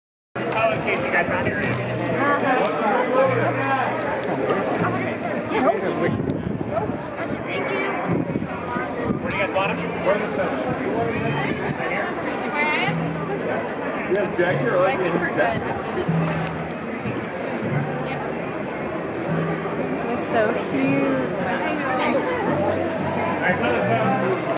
Dreamworks Animation Kung Fu Panda 4 World Premiere The Grove Los Angeles California March 3, 2024
Dreamworks Animation Kung Fu Panda 4 World Premiere The Grove Los Angeles California USA March 3, 2024